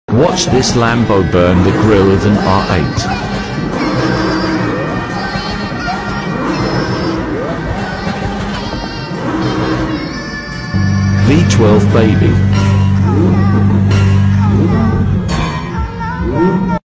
Lamborghini burnout grill of Audi r8
lambo exhaust firing up and burning front grill of Audi r8 standing behind.